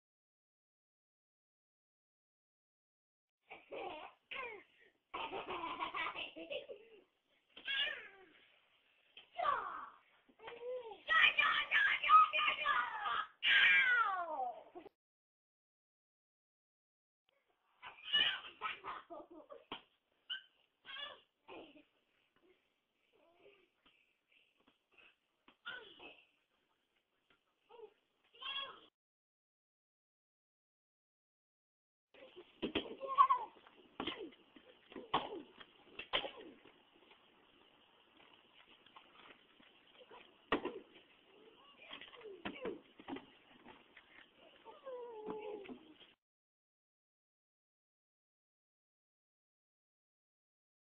Camden Beating up Noah
Camden giving Noah a beating with a bat!